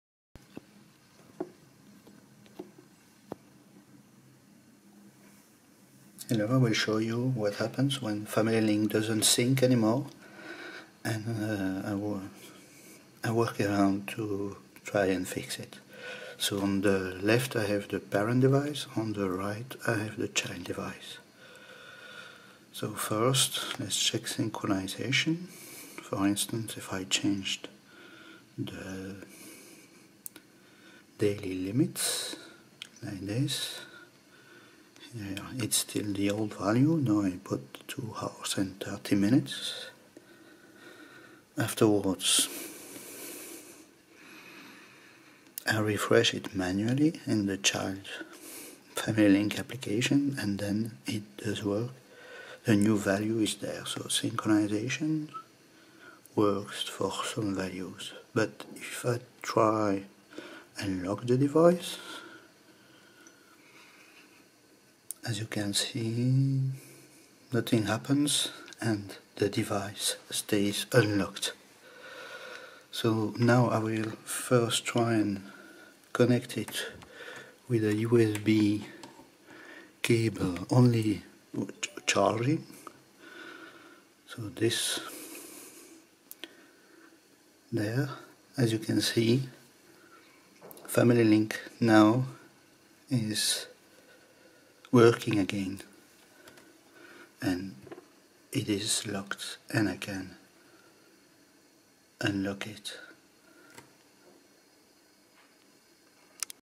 Google Family Link notifications do not work : try this workaround
Google Family Link application does not display push notifications after installation. "Lock screen" does not work, neither does "play sound", newly blocked applications are still allowed, activity counters are not increasing, ...
The workaround is to plug the phone with a USB cable to let it charge for a few seconds, and Family Link starts fully working.  Afterwards, it keeps working even after phone reboots most of the time, but rarely, Family Link notifications are missing again, and the solution is again to plugin the phone with a USB cable...
Note : a solution suggested by another person is to add the Family Link application to the list of "power-saving exceptions".  I tried adding Family Link (and Google Play Services was present by default in the list), but it did NOT restore the Family Link notifications.  I also added all possible applications and system packages (more than 300) in the exception list, but that did not help either.